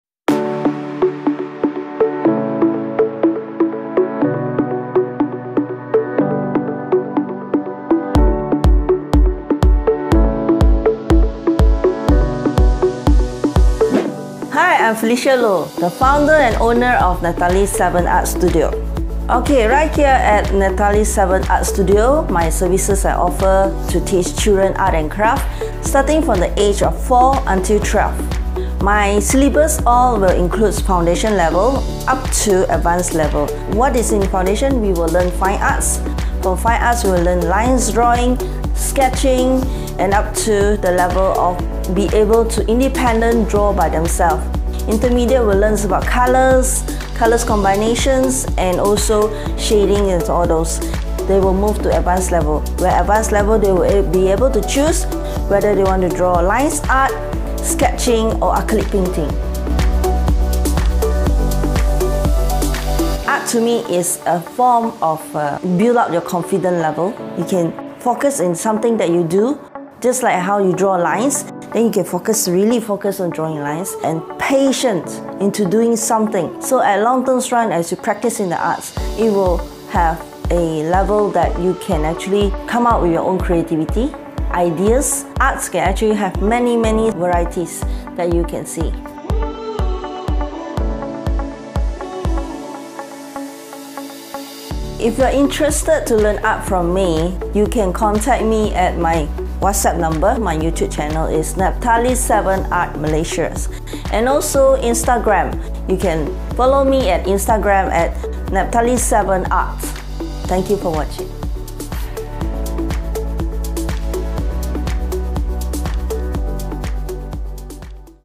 Felicia Loh - Founder and Owner of Naphtali 7 Art Malaysia
Felicia Loh is the founder and owner of Naphtali 7 Art in Malaysia. A talented artist whose skills gleam beyond. With a heart made of love, she carries with her an unswerving message of confidence through creativity. ICF Coach, Specialise in Developing Moto Skill, Mind Focus, Self-Confident and Expression.